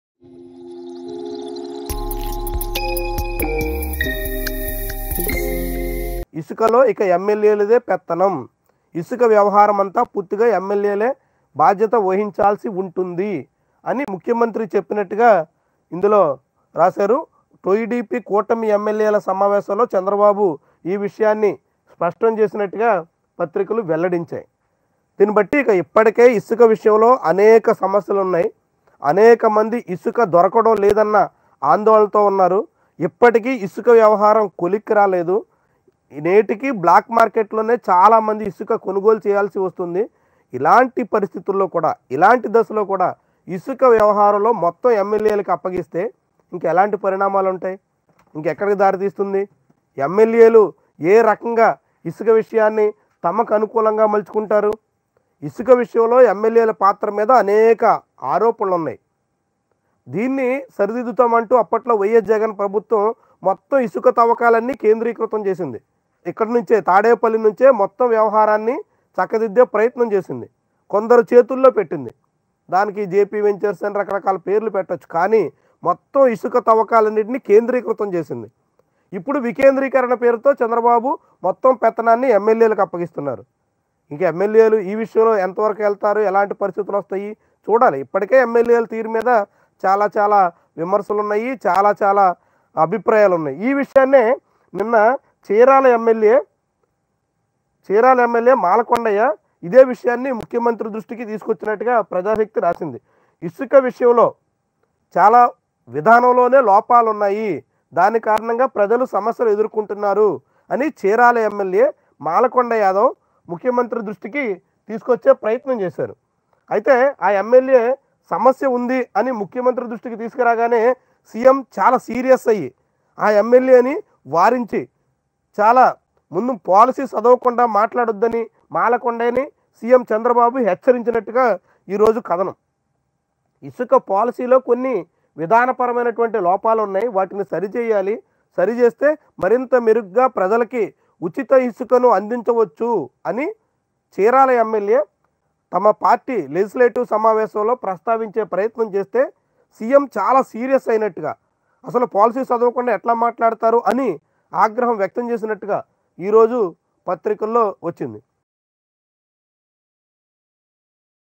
Andhra Pradesh: ఇసుక బాలేదని చెప్పినందుకు ఎమ్మెల్యేపై సీఎం చంద్రబాబు కన్నెర్ర!
ఏపీలో ఇసుక విధానం మీద పలు విమర్శలున్నాయి. అనేక మంది పెదవి విరుస్తున్నారు. అదే విషయాన్ని పాలక టీడీపీకి చెందిన చీరాల ఎమ్మెల్యే సీఎం దృష్టికి తీసుకెళ్లే ప్రయత్నం చేశారు. కానీ సీఎం ఆయన మీద ఆగ్రహం వ్యక్తం చేశారు. కొండయ్య యాదవ్ ను ఘాటుగా హెచ్చరించారు.